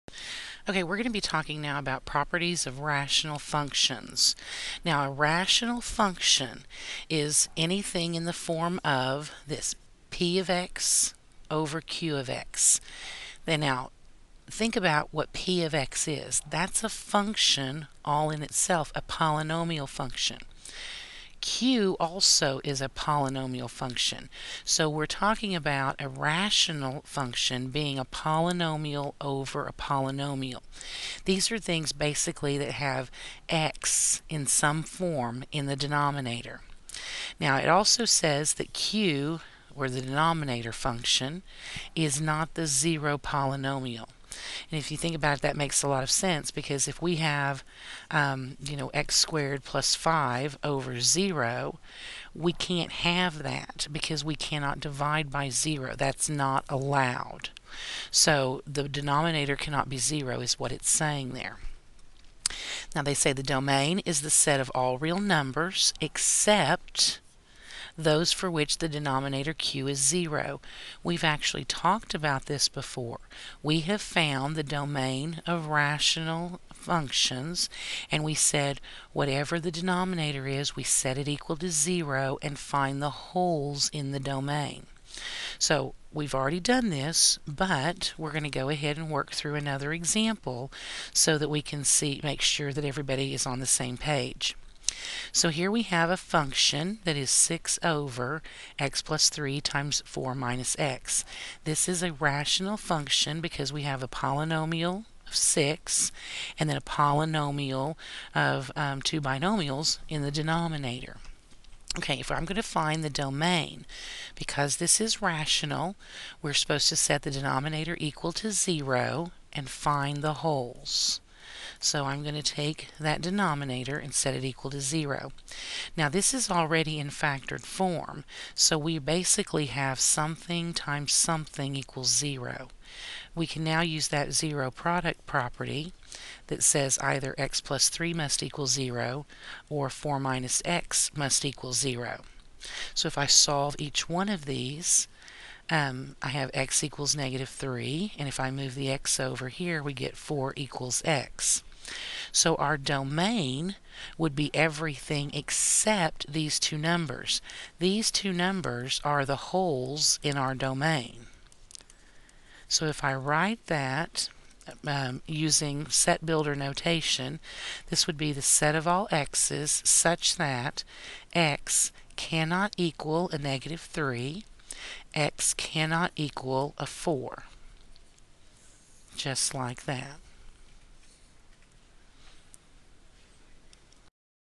Domain of a Rational Function 5.4
How to find the domain of a rational function.